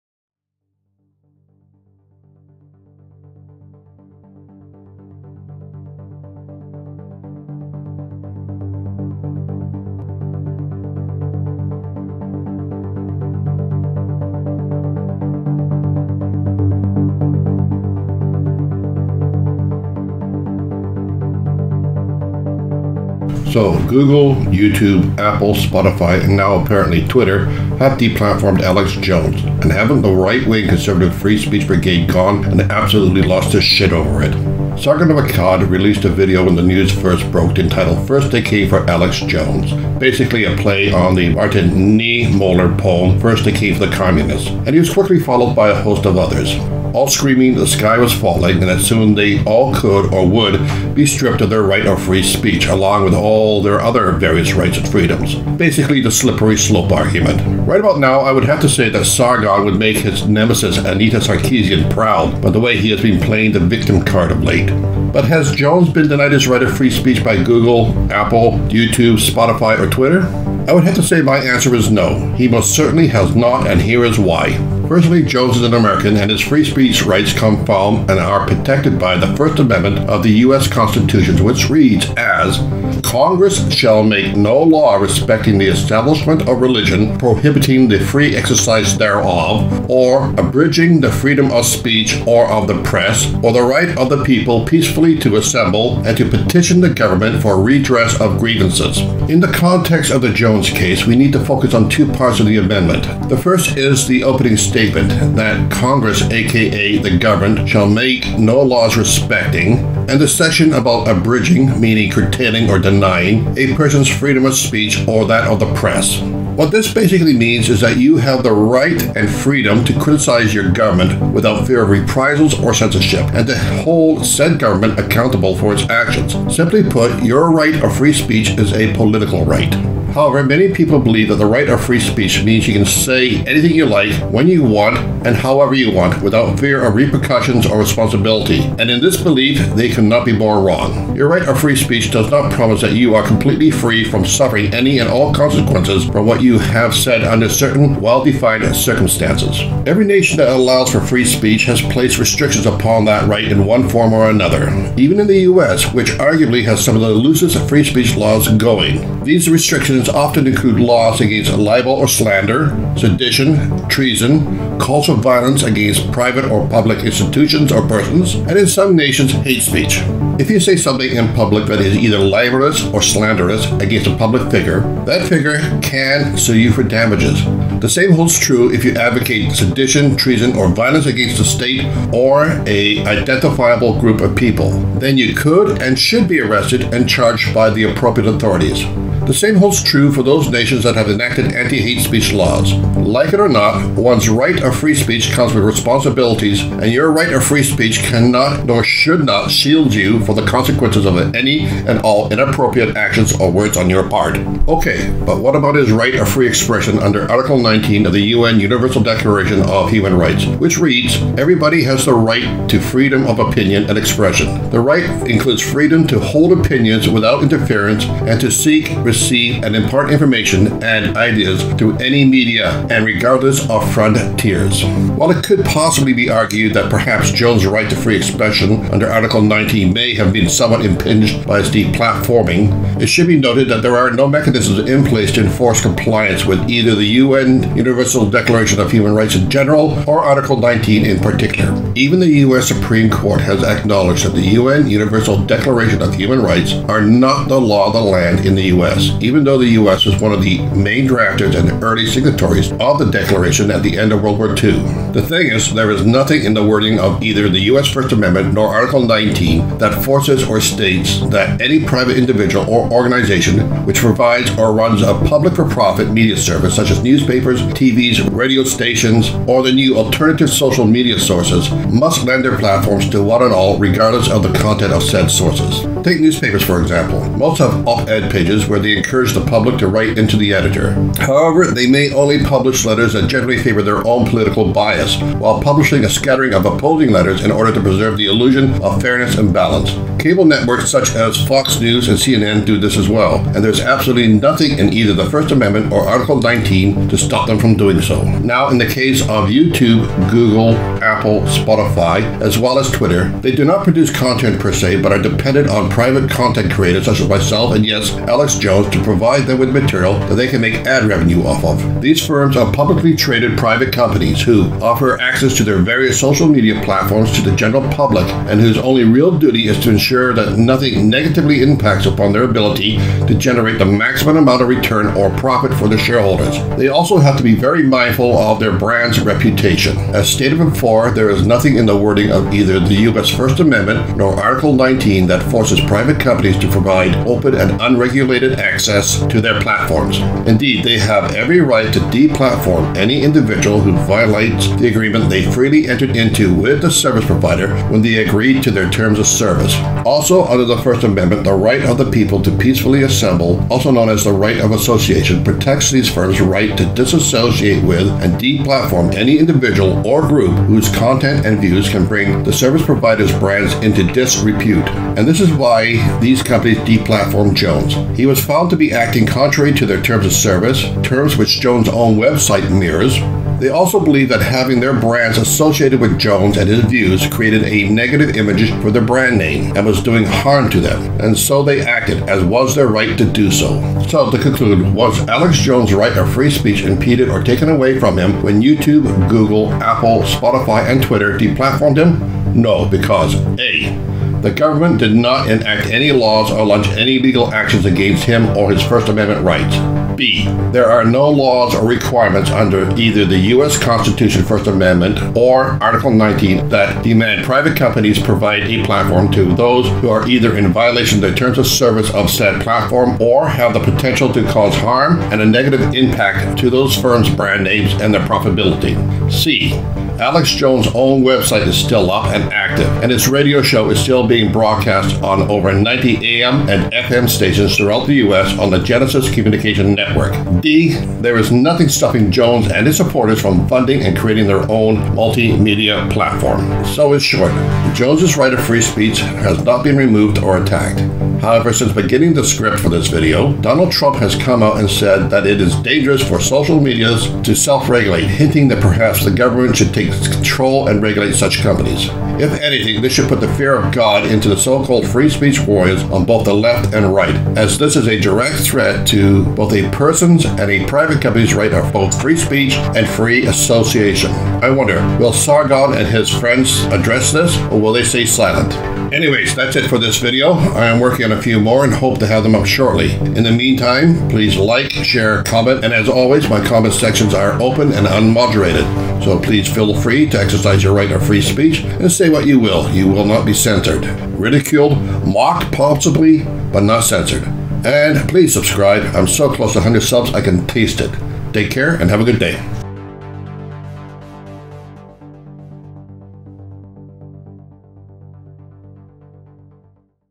Free Speech & Alex Jones (Hint, his weren't taken away or threatened)
Today we tackle Alex Jones and free speech, a subject I have been thinking of covering for a while now.   As always, the comment section is open and unmoderated, feel free to exercise your right of free speech here, you will not be censored, however I cannot nor will not promise you will not be ridiculed or mocked.